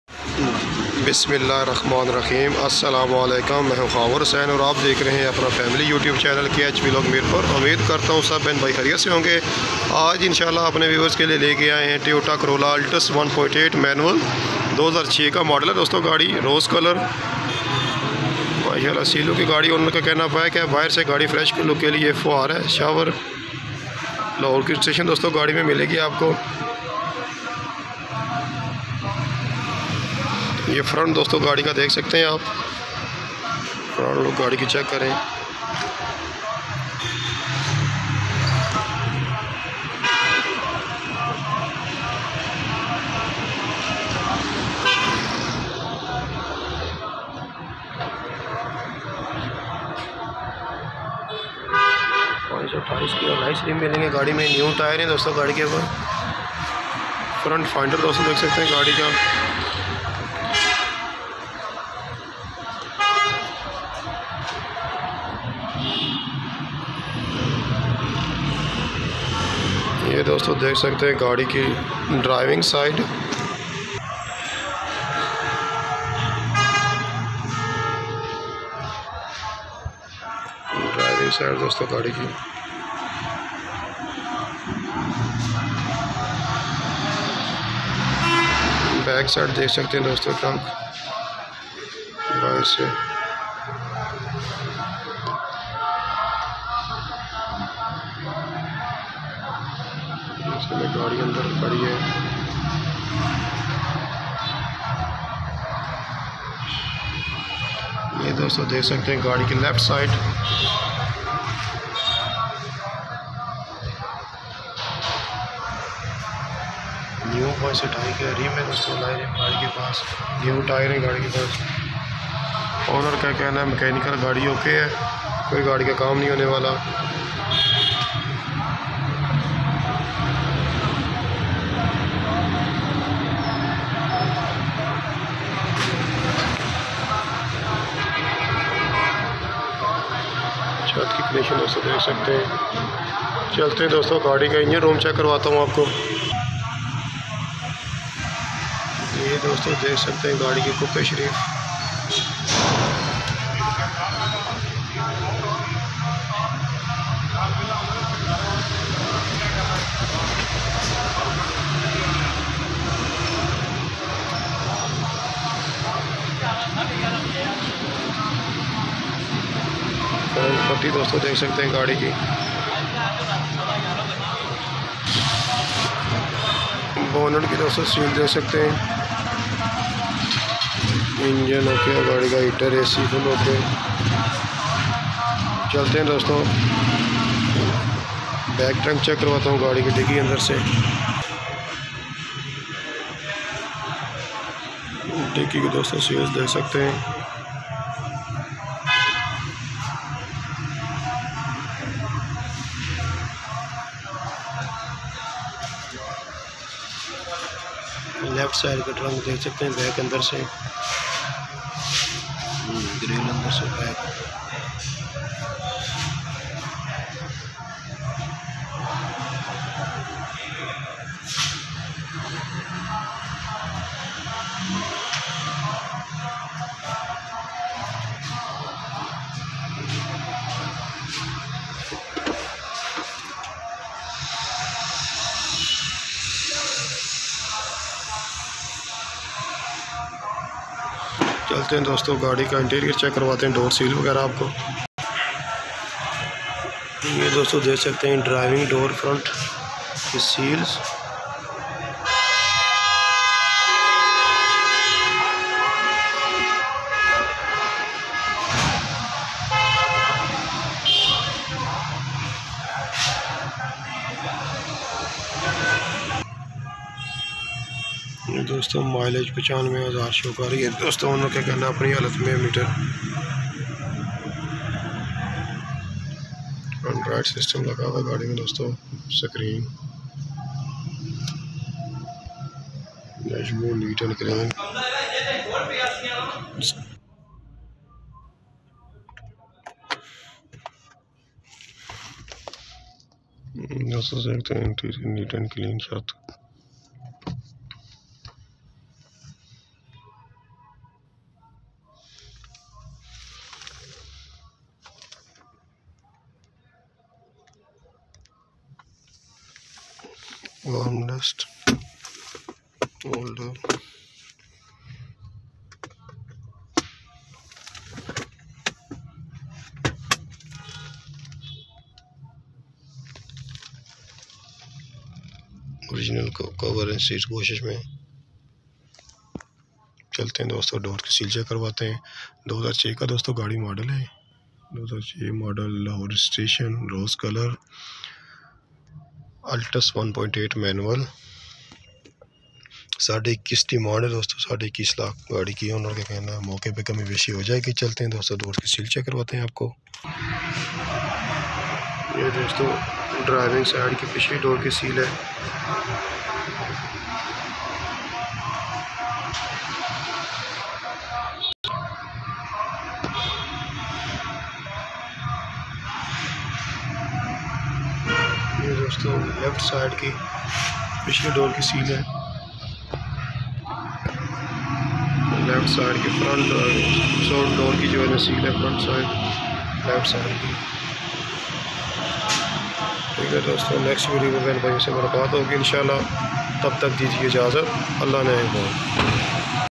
Toyota Corolla Alts 1.8 manual used car for sale in Pakistan Ajk full review
Toyota Corolla Alts 1.8  manual
model 2006
colour Rose matlic
regstriton lahour
condition inner totally seel to seel out side shower for fresh look
location Mirpur Azad kashmir